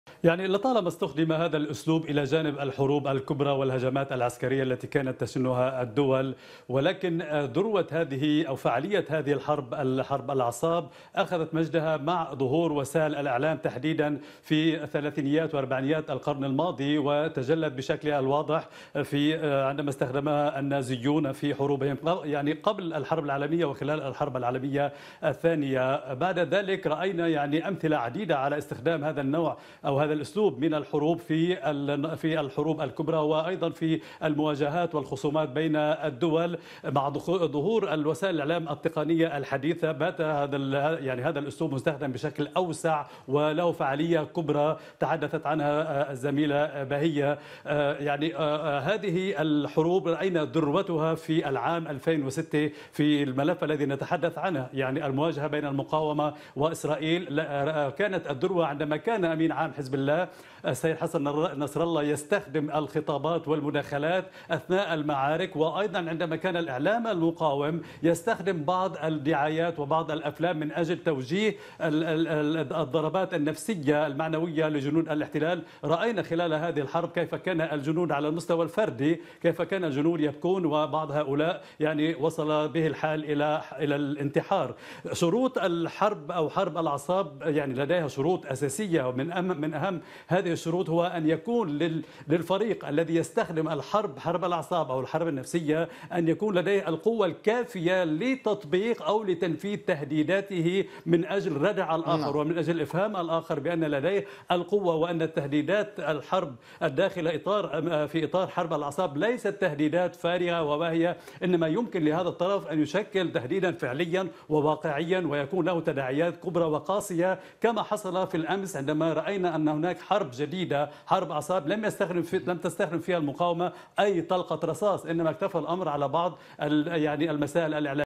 عاصي: لحرب الاعصاب شروط أساسية أهمها القوة
"حرب أعصاب.. انتظروا على إجر ونص" كتبت "يديعوت احرونوت".. هي حرب أعصاب في توصيفها لما جرى يوم أمس.. ما عناصر هذه الحرب، وأي دور للإعلام التقليدي والحديث فيها؟ وكيف نجحت الأخيرة في إتلاف أعصاب "إسرائيل"؟